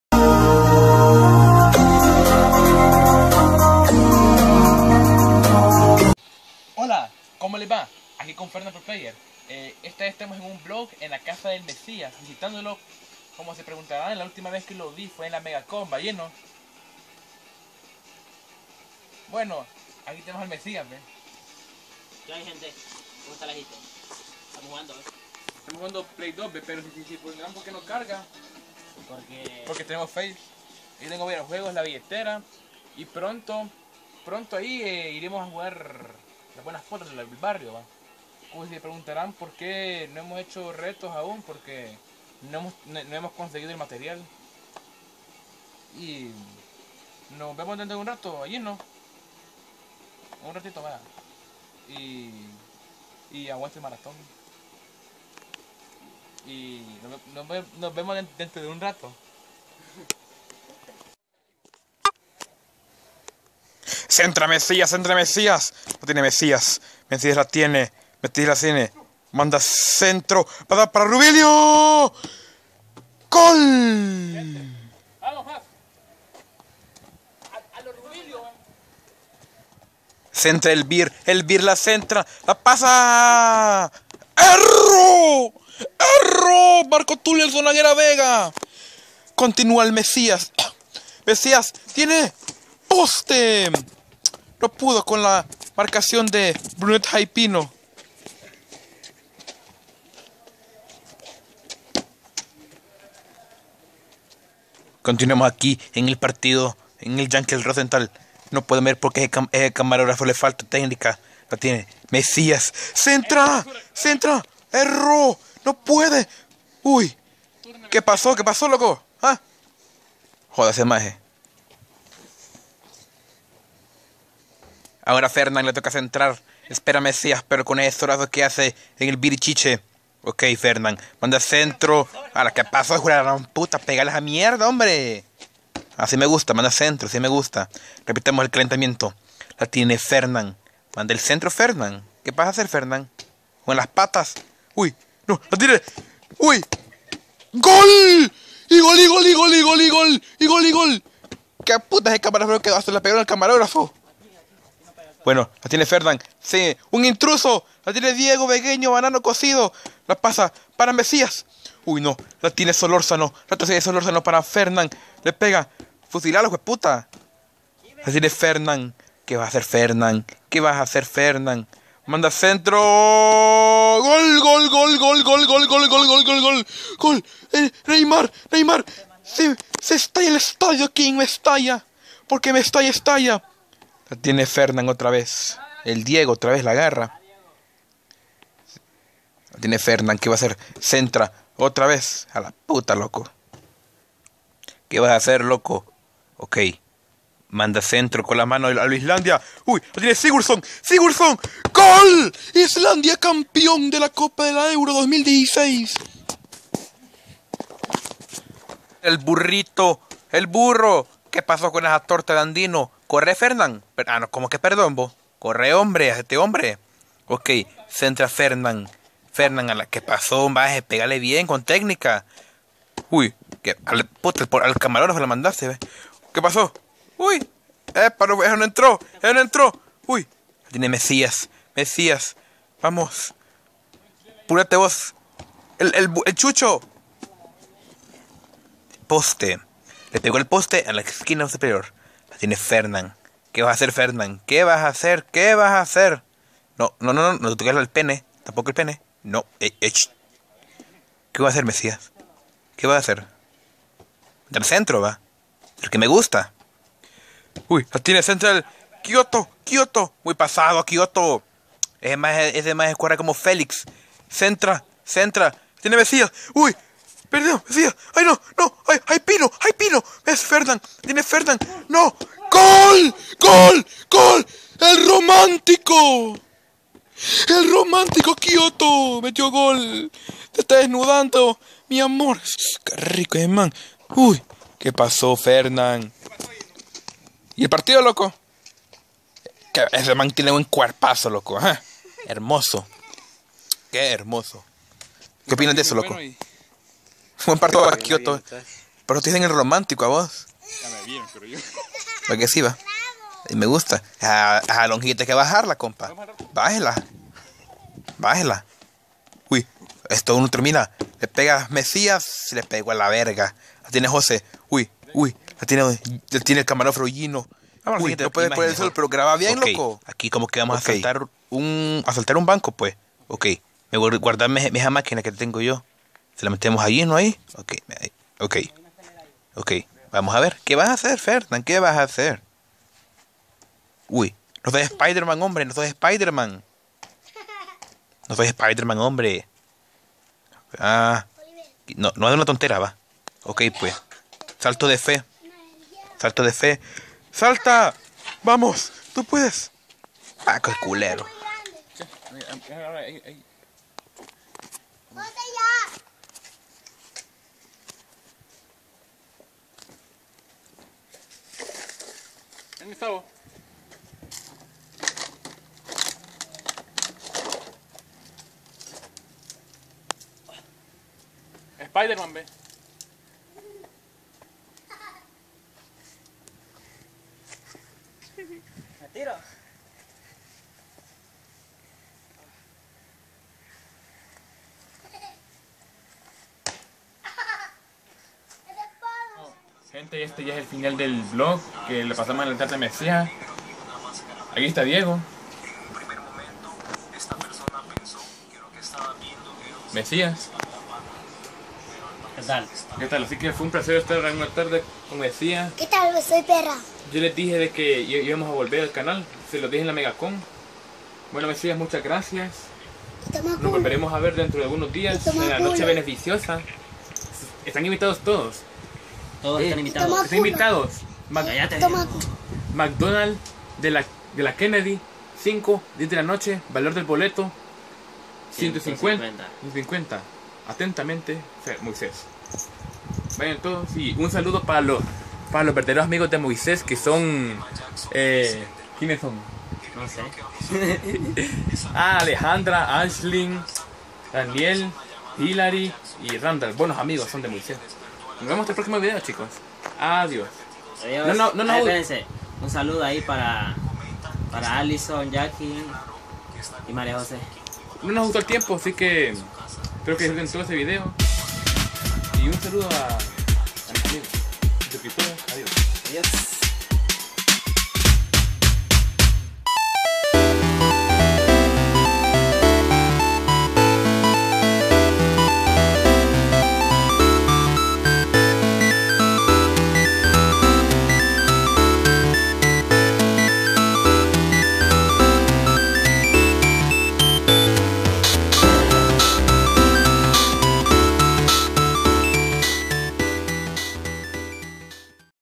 UNA TARDE CON MESSIAS Y GINO
Hola gente de youtubermaniacos esta vez es el video de la tarde com messias y espero que les guste y den like y arriba el motagua